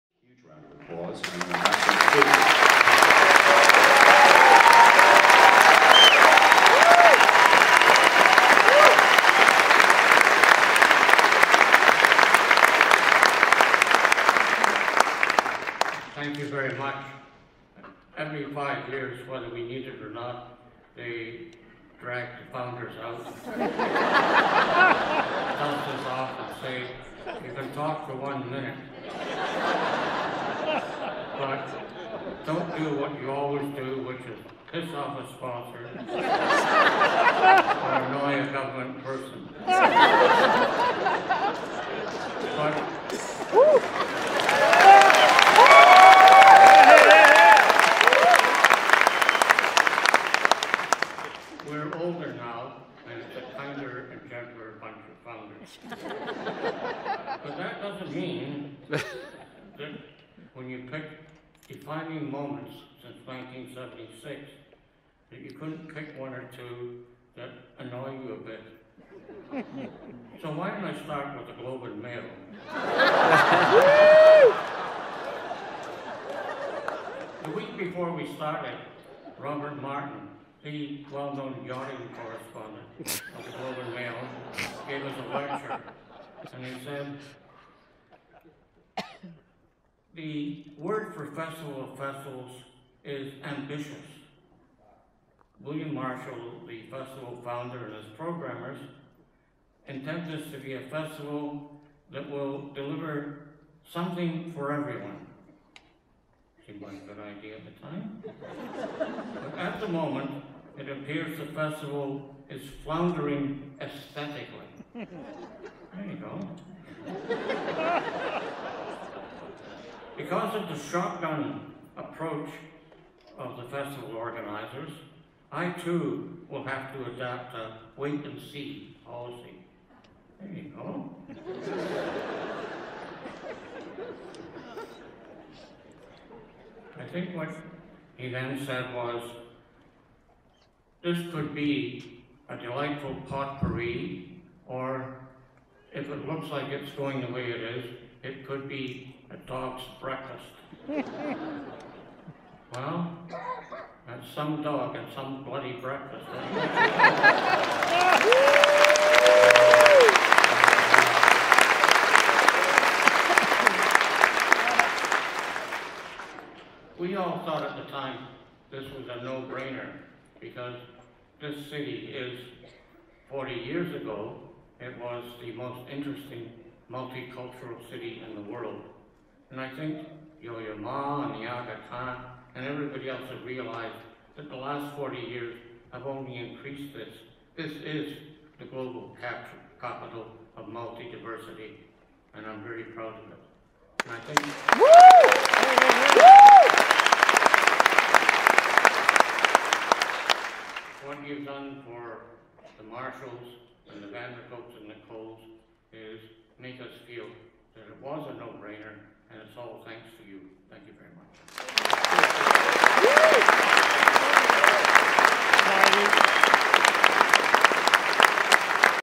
TIFF Founder Bill Marshall pokes fun at the Globe & Mail for early press coverage for festival.